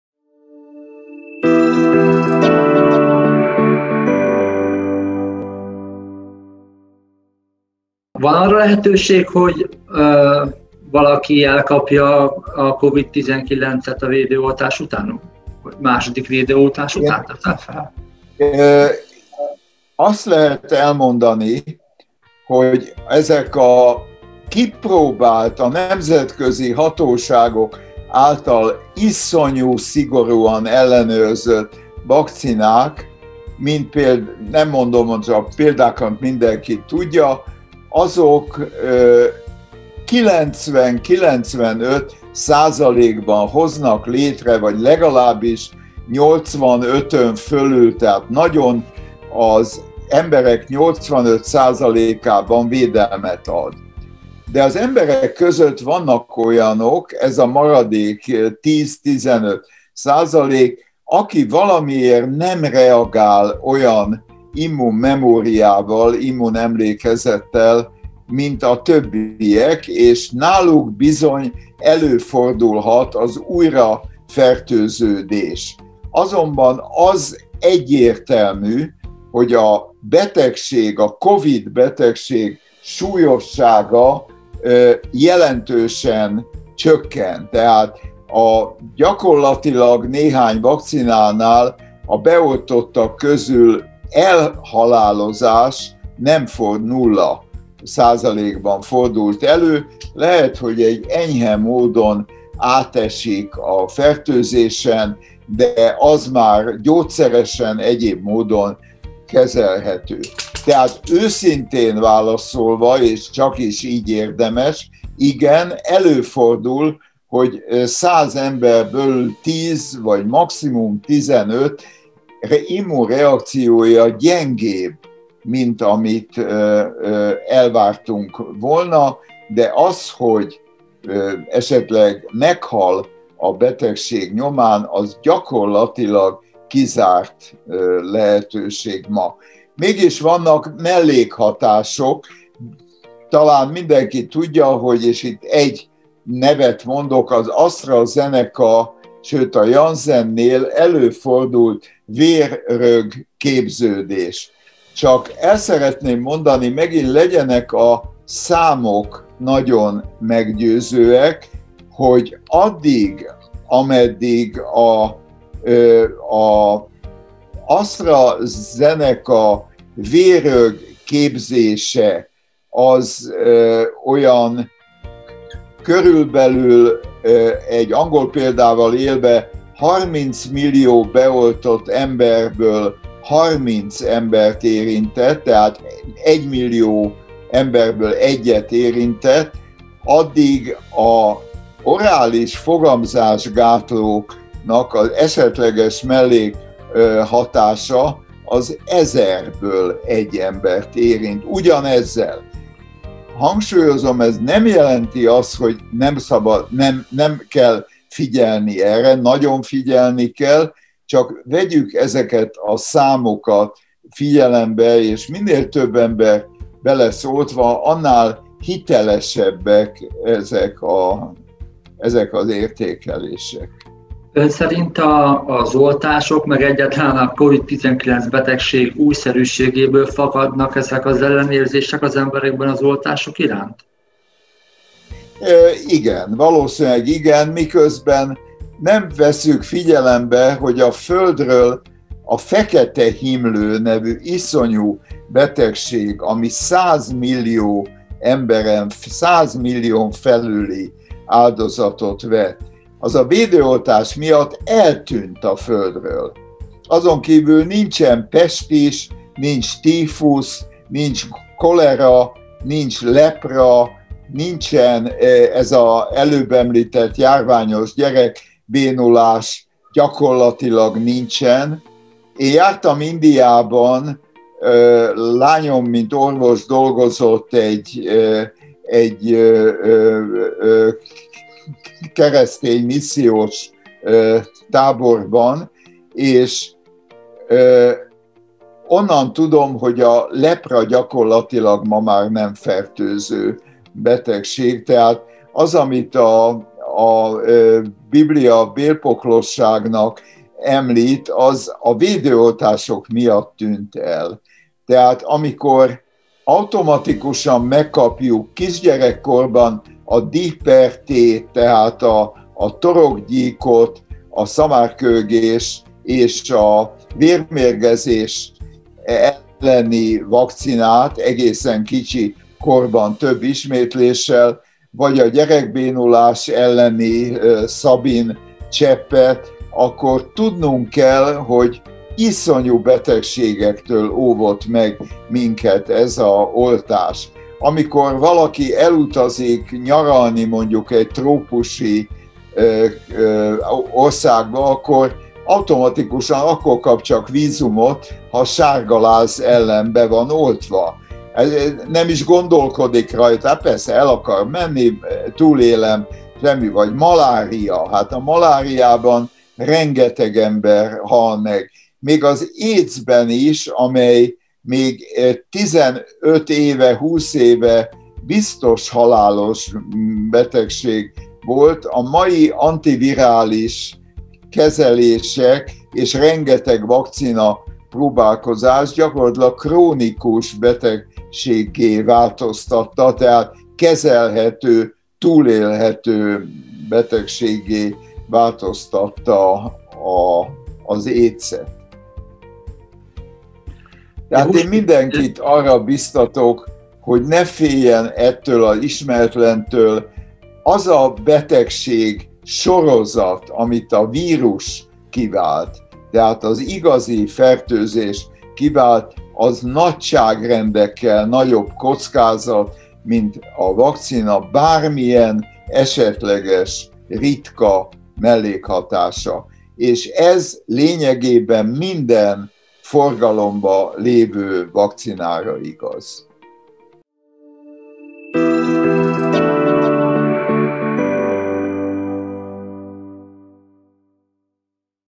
El lehet-e kapni a Covid-19-et az oltás után is? - Válaszol: Prof. Falus András immunológus
Református gyülekezeti tagok és szakemberek segítségével igyekszünk a Covid-19-ként ismert koronavírus elleni védőoltás beadatására bátorítani követőinket. 💉

Az országszerte ismert Széchenyi-díjas immunológus, Falus András, a Semmelweis Egyetem professor emeritusa a vele készített videointerjúnk nyolcadik részében arra válaszolt, hogy el lehet-e kapni a Covid-19-et a védőoltás után is. 🙏🏼

Az oltás életeket ment - oltasd be magad és bátoríts erre másokat is! 😎